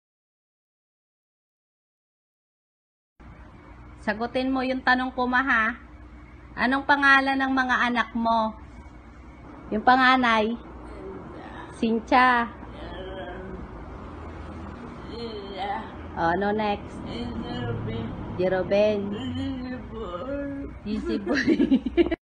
STROKE PATIENT MULING NAKAPAGSALITA
Isa namang Amazing Testimonial mula sa Nanay ni Ptra Cynthia after  taking 11 Sachets of STC30

Blessed na blessed ang aming pamilya nakakapagsalita na ulit ang aming ina. Last August na stroke po si Mama Hindi na nakapagsalita puro unggol, at iyak dahil di nya masabi ang mga gusto nya. Hirap ang kalooban namin magkakapatid Lalo na si Papa dahil Sila ang laging magkasama, ang mga kamag anak, kaibigan at mga kapit bahay namin awang awa kay Mama. Purihin ang Panginoon narinig ko sa isang churchmate ko ang Stem Cell Theraphy STC30 fruit stem cell po ito. Sa testimony ng churchmate ko sya ay HIV-Aids positive ito po ang nagpagaling sa kanya. Dahil sa narinig ko di po kami nag atubili na bumili. Pinainom po namin si Mama first week of December ang prayer namin ng kapatid kong si Lhea bago mag Christmas makakapagsalita na si Mama. At nangyari po yon. 11 sachet lang po ang nainom ni Mama nakapag salita na po sya, di na antokin gaya ng dati in 5 minutes lang syang nka upo tulog na,at ang dating maputla nyang kulay ngayon po ay glowy na. Glory be to God and sa STC30 Nabanggit na ni Mama ang pangalan ng mga anak nya. Cynthia, Carol,Lhea, Jerobin,Jessieboy, yon nga lang bahagyan ko syang napaiyak haha after sabihin ang pangalan ng Bunso namin umiyak sya. Miss na Miss nya na daw si Manager Eboy.

Sa mga may karamdaman pwd nyo po ako i message. I will help you po kung San makakabili ng Stem cell STC30.